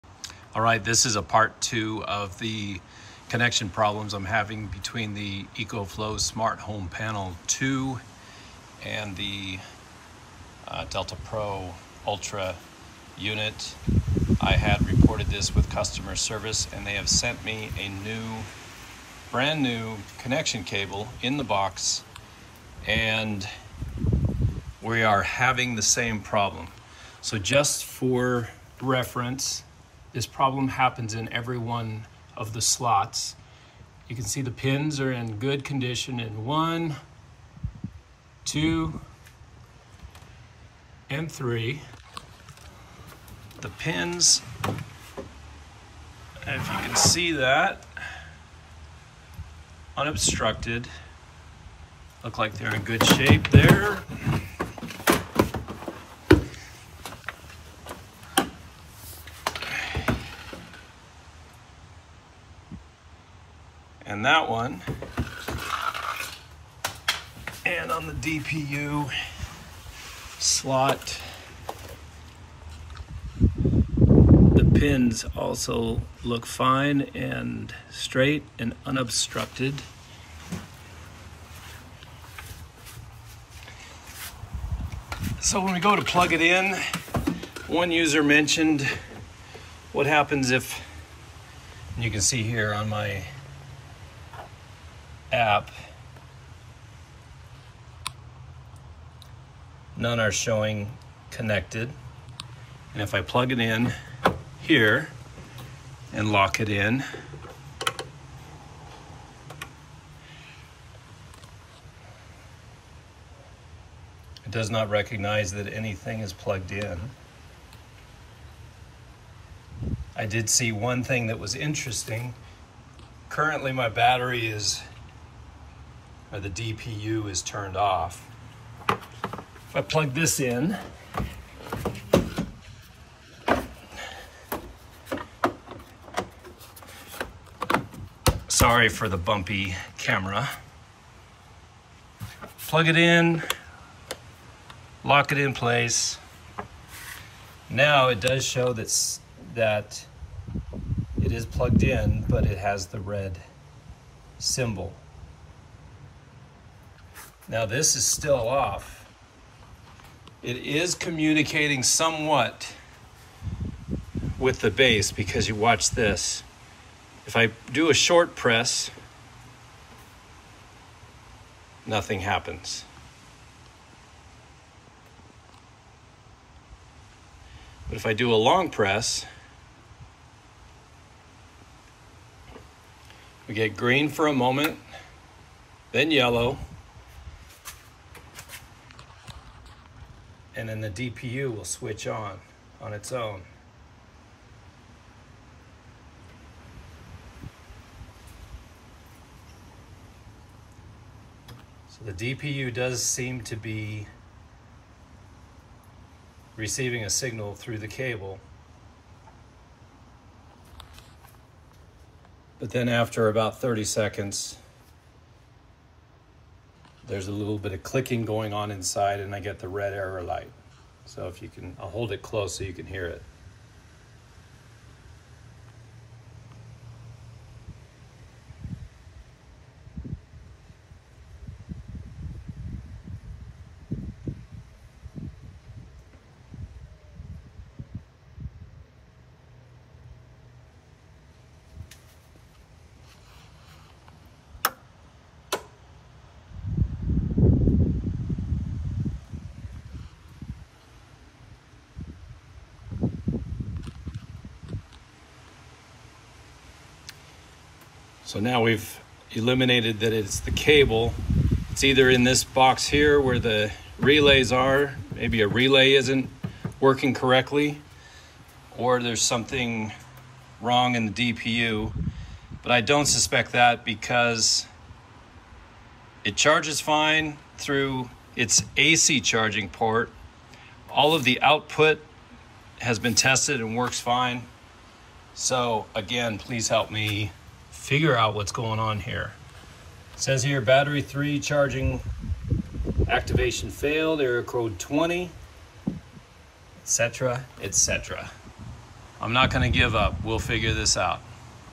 EcoFlow Smart Home Panel 2 and Delta Pro Ultra connection problems. Follow-up video with new cable.
This is a follow-up video to my previous. EcoFlow sent me a new cable, thinking that the cable was the problem. Here is the test with the new cable. The firmware for both SHP2 and DPU are up to date.